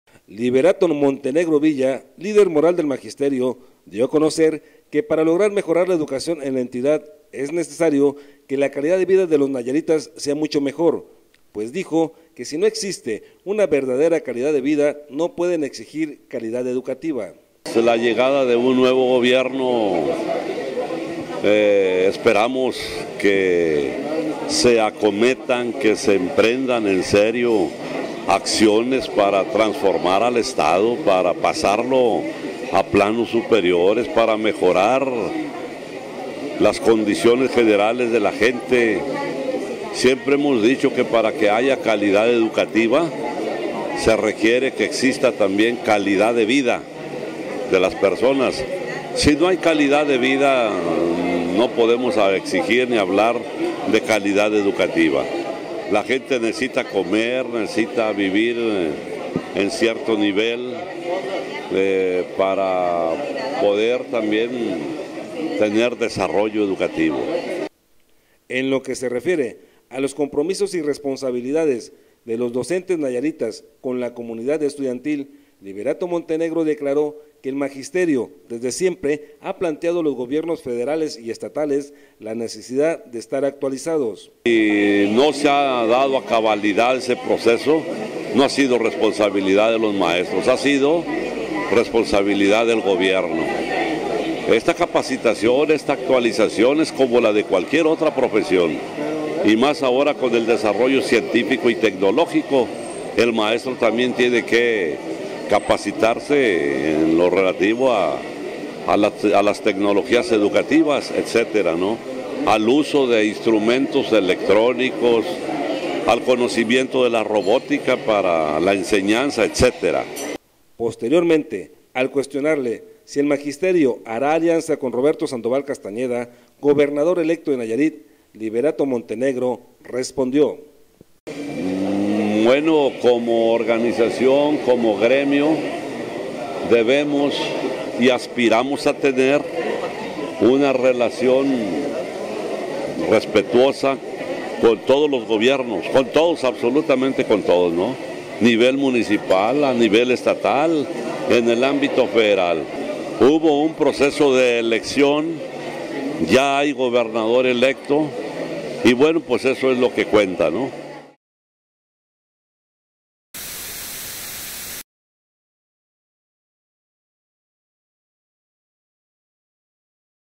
Espera el magisterio respeto del gobierno de Roberto
Como organización, como gremio, debemos y aspiramos tener una relación respetuosa con todos los gobiernos, absolutamente con todos, nivel municipal, a nivel estatal y en el ámbito federal, resaltó el líder moral del sector, Liberato Montenegro Villa